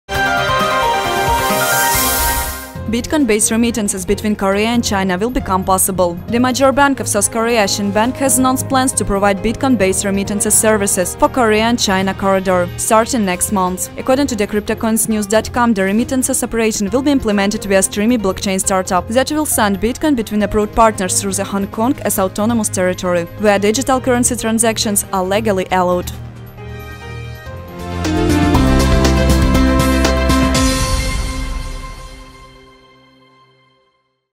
KCN News: Shinhan Bank will launch Bitcoin remittances
Bitcoin-based remittances between Korea and China will become possible. The major bank of South Korea Shinhan Bank has announced plans to provide Bitcoin-based remittance services for the Korean-China corridor, starting next month. According to the Cryptocoinsnews.com, the remittance operations will be implemented via Streami blockchain-startup, that will send Bitcoin between approved partners through the Hong Kong as autonomous territory, where digital currency transactions are legally allowed.
EDR: anna-profit
BTC: 1JdRXQuqicwSyn61cd9jyXzAk2QUfu4KEW